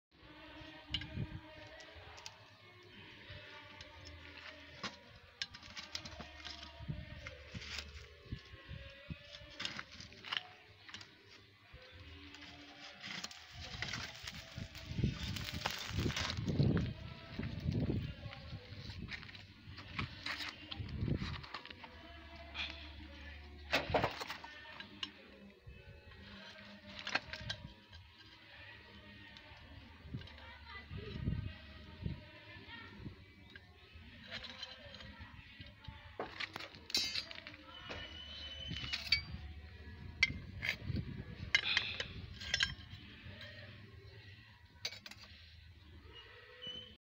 casting the Gingery lathe compound swivel base
anxious to own a lathe  machine of my own Diy through Gingery metal lathe series